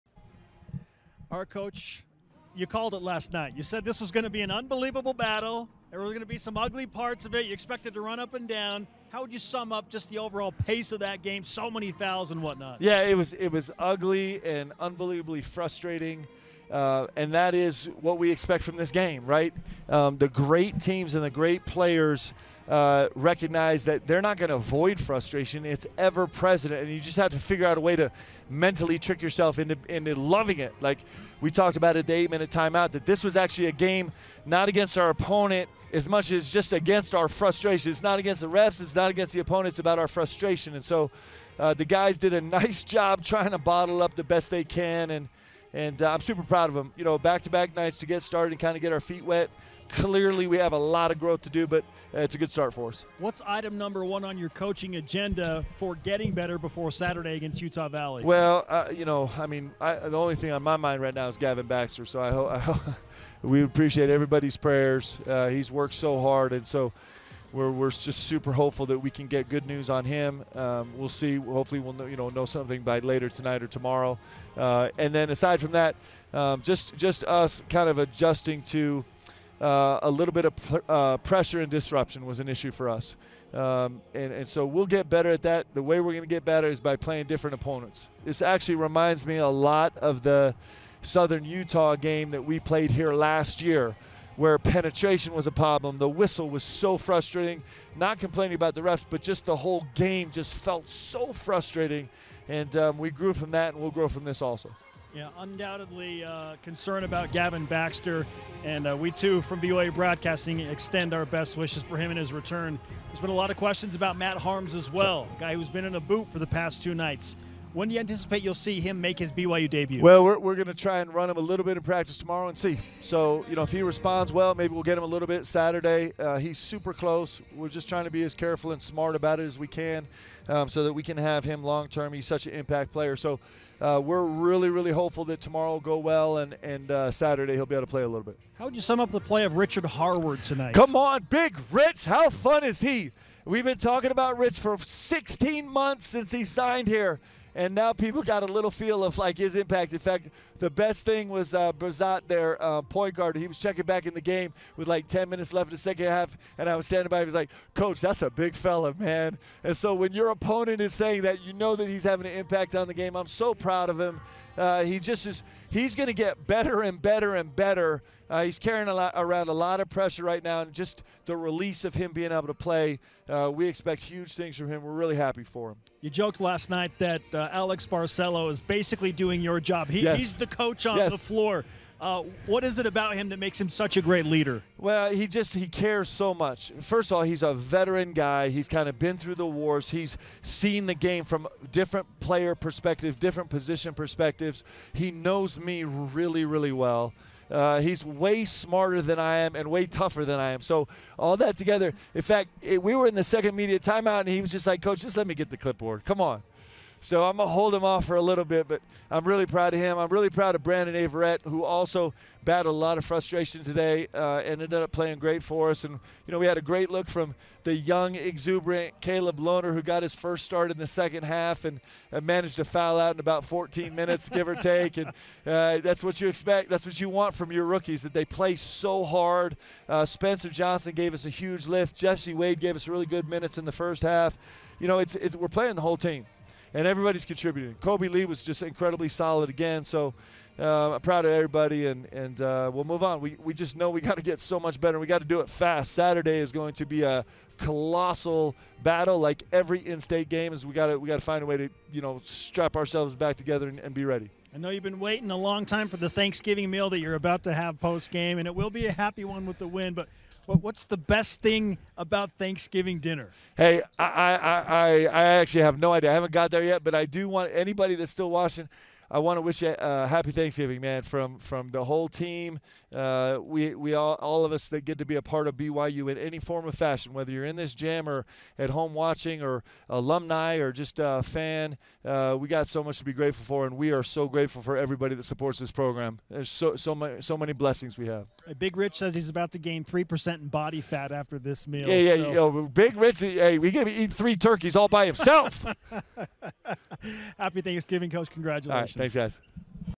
Mark Pope Postgame Interview BYU vs. New Orleans
Coach Mark Pope comments on BYU Basketball's "ugly and frustrating" win over New Orleans, and he couldn't be more proud of his guys.